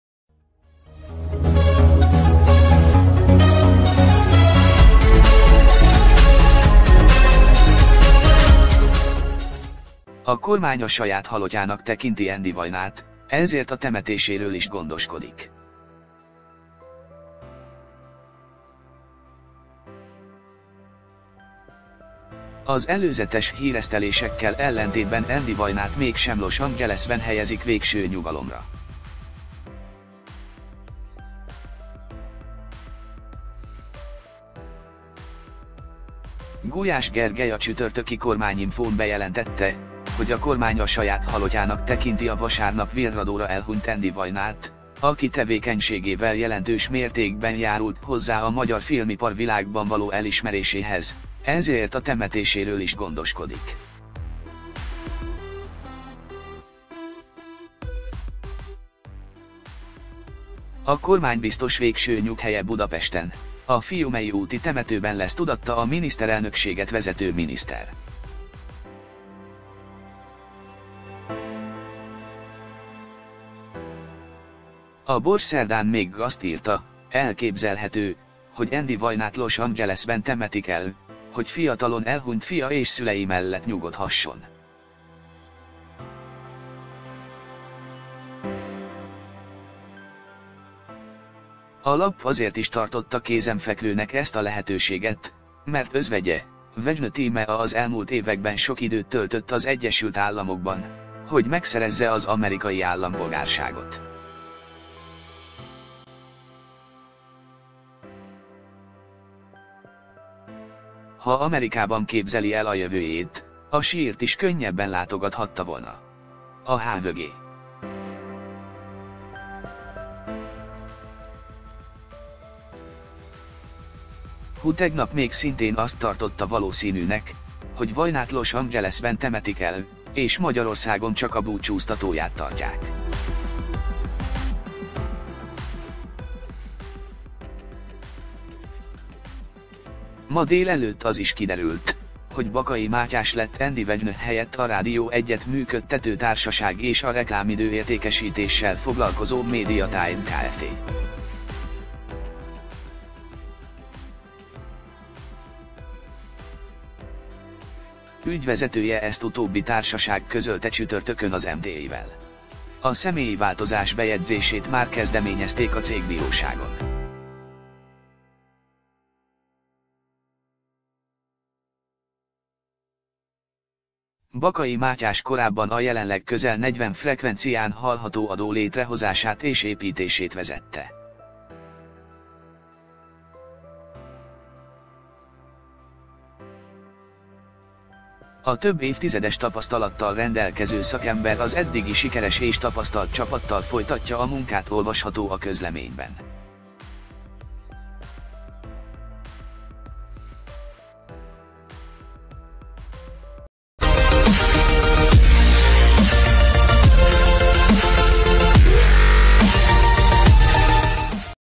Andy Vajna sírja nem a fiáé mellett lesz - Most jelentették be, hova temetik
A kormány a saját halottjának tekinti Andy Vajnát, ezért a temetéséről is gondoskodik.
Az előzetes híresztelésekkel ellentétben Andy Vajnát mégsem Los Angeles-ben helyezik végső nyugalomra. Gulyás Gergely a csütörtöki kormányinfón bejelentette, hogy a kormány a saját halottjának tekinti a vasárnap virradóra elhunyt Andy Vajnát, aki tevékenységével jelentős mértékben járult hozzá a magyar filmipar világban való elismeréséhez, ezért a temetéséről is gondoskodik
A kormánybiztos végső nyughelye Budapesten, a Fiumei úti temetőben lesz - tudatta a Miniszterelnökséget vezető miniszter.A Bors szerdán még azt írta, elképzelhető, hogy Andy Vajnát Los Angeles-ben temetik el, hogy fiatalon elhunyt fia és szülei mellett nyugodhasson. A lap azért is tartotta kézenfekvőnek ezt a lehetőséget, mert özvegye, Vajna Tímea az elmúlt években sok időt töltött az Egyesült Államokban, hogy megszerezze az amerikai állampolgárságot. Ha Amerikában képzeli el a jövőjét, a sírt is könnyebben látogathatta volna. A hvg.hu tegnap még szintén azt tartotta valószínűnek, hogy Vajnát Los Angeles-ben temetik el, és Magyarországon csak a búcsúztatóját tartják.
Ma délelőtt az is kiderült, hogy Bakai Mátyás lett Andy Vajna helyett a Rádió 1-et működtető társaság és a reklámidő-értékesítéssel foglalkozó Media Time Kft. ügyvezetője - ezt utóbbi társaság közölte csütörtökön az MTI-vel. A személyi változás bejegyzését már kezdeményezték a cégbíróságon.Bakai Mátyás korábban a jelenleg közel negyven frekvencián hallható adó létrehozását és építését vezette. A több évtizedes tapasztalattal rendelkező szakember az eddigi sikeres és tapasztalt csapattal folytatja a munkát - olvasható a közleményben.